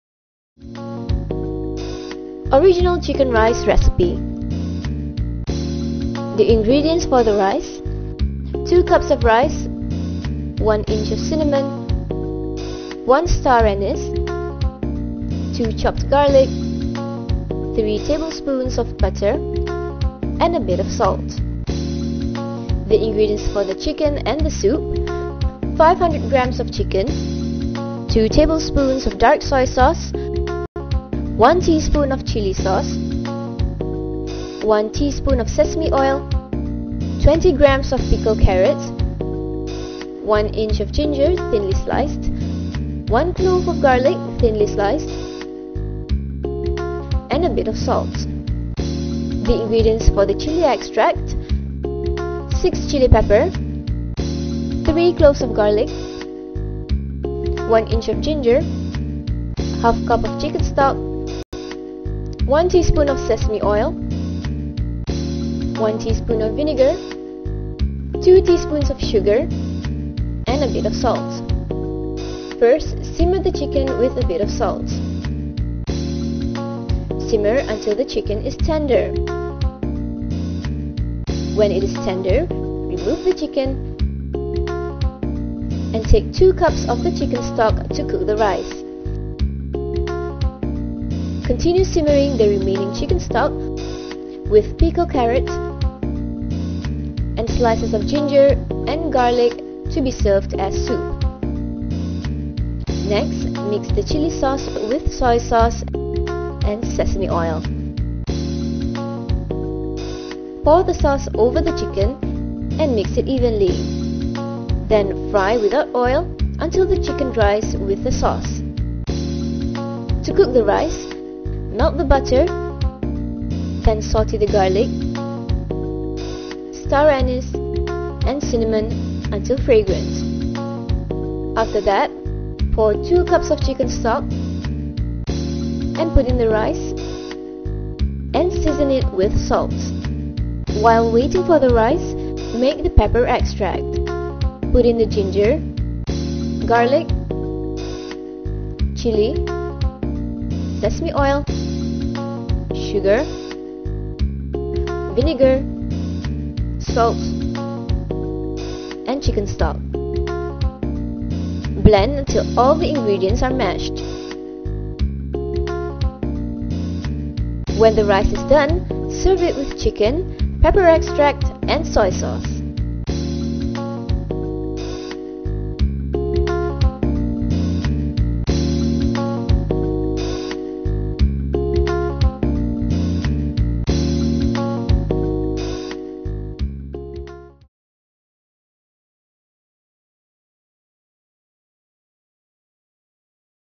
Original Chicken Rice
Here's a simple way to make Original Chicken Rice. Do enjoy the video :)